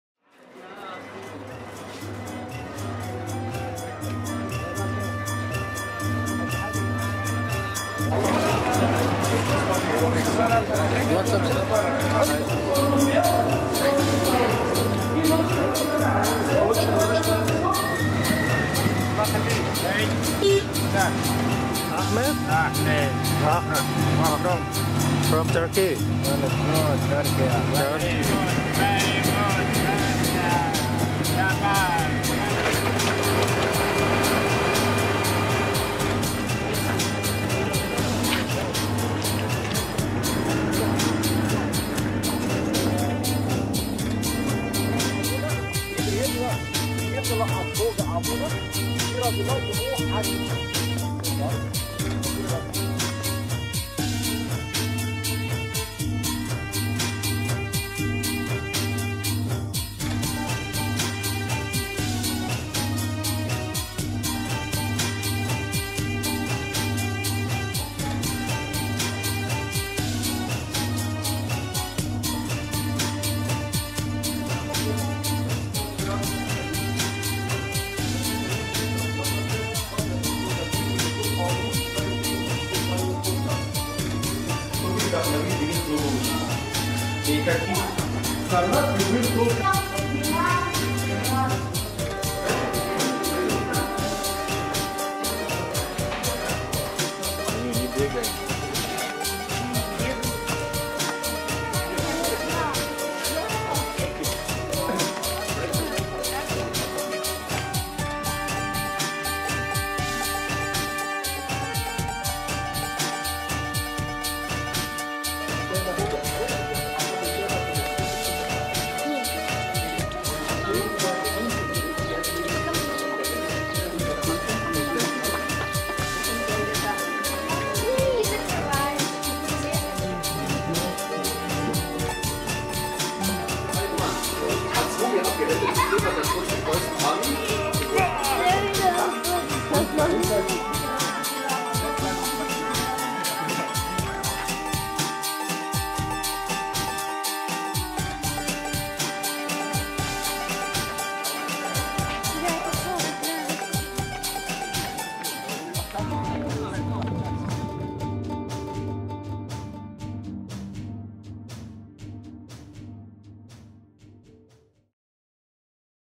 Hatcepsut Tapınağı, Luksor
Hatşepsut, Antik Mısır'da, 18. Hanedanın altın çağında (MÖ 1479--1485) hüküm sürmüş, tarihin gördüğü ilk ve tek kadın firavundu. Bir erkek rolünü üstlenme cesaretine sahip oluşuyla ünlenmiş sıradışı Kraliçe Hatşepsut, üvey oğlunu gölgede bırakıp, Eski Mısır'ı bir erkek gibi yönetti.